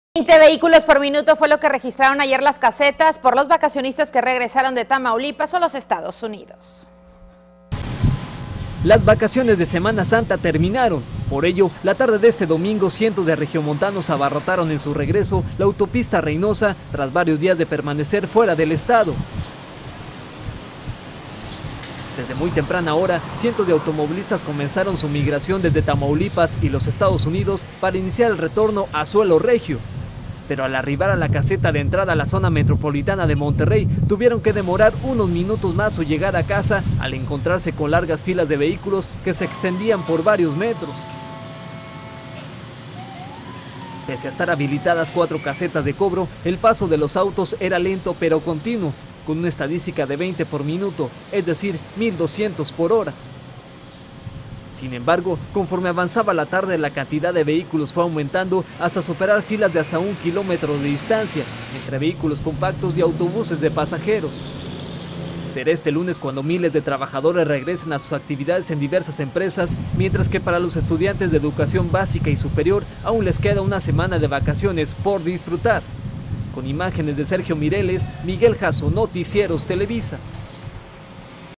Las Noticias - Regresan vacacionistas; abarrotan autopista a Reynosa
La Autopista a Reynosa también lució abarrotada. Desde temprana hora, largas filas de vehículos se formaron en las casetas